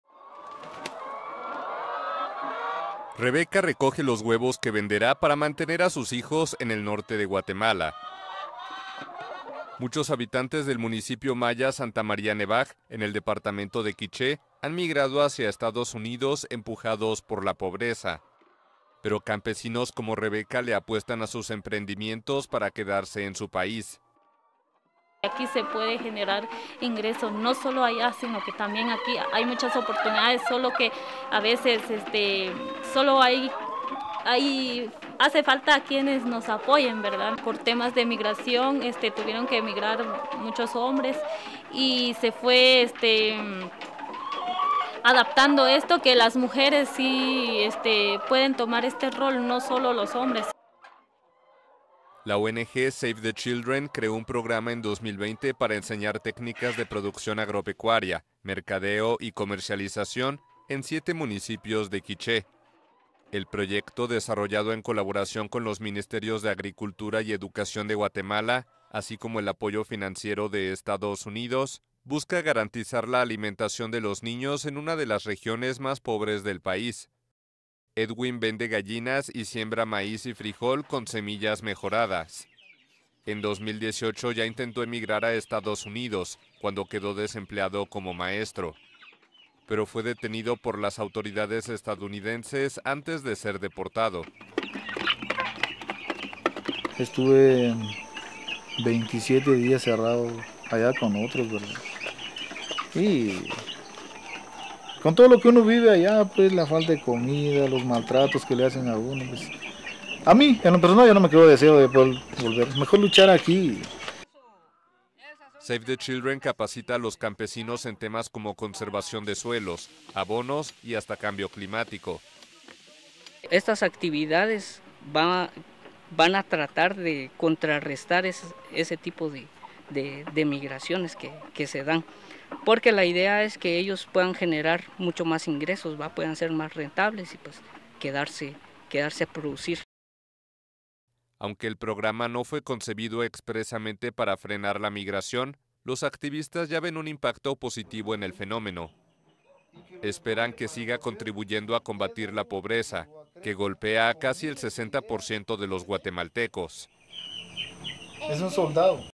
Gallinas y cultivos, una apuesta de indígenas guatemaltecos contra la migración
Las gallinas revolotean en el corral mientras Rebeca recoge los huevos que venderá en su aldea en el norte indígena de Guatemala, un emprendimiento al que apuesta para no emigrar a Estados Unidos como lo hicieron sus dos hermanos, empujados por la pobreza.